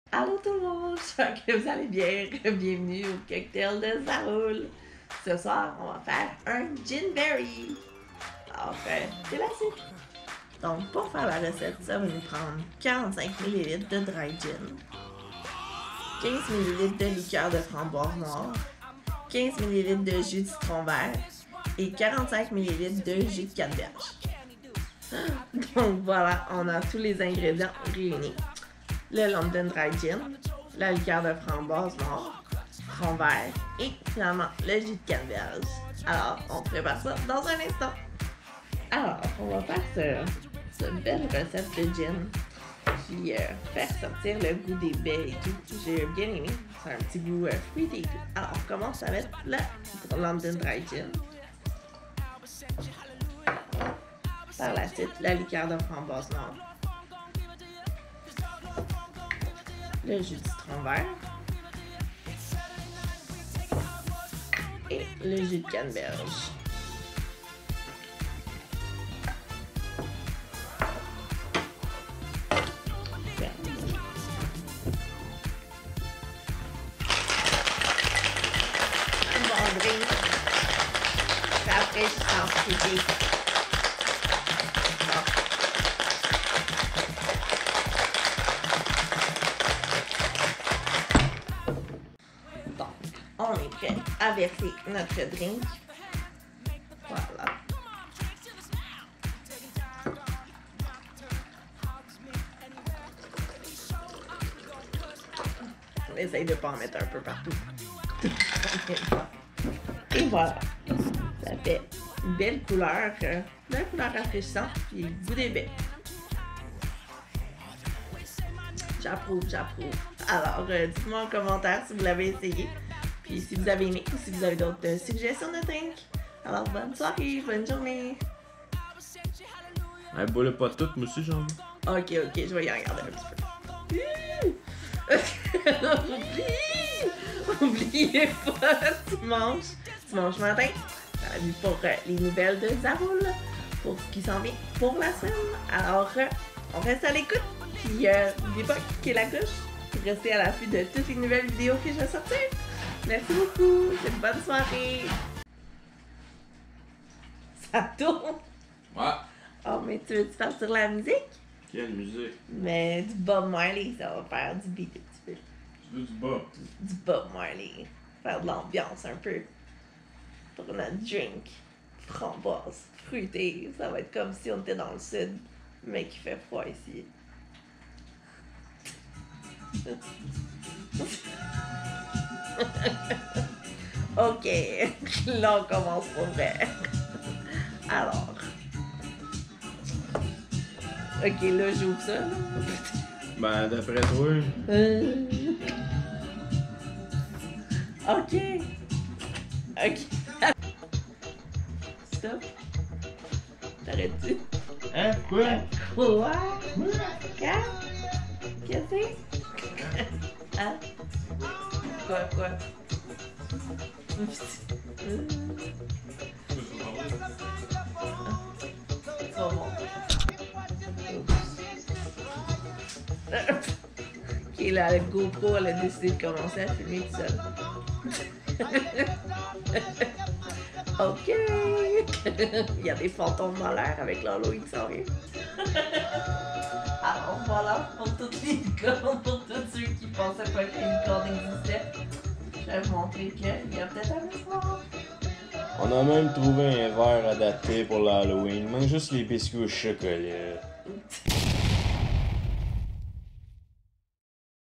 Gin Berry
Aujourd'hui avec Zaroule, le cocktail Gin Berry!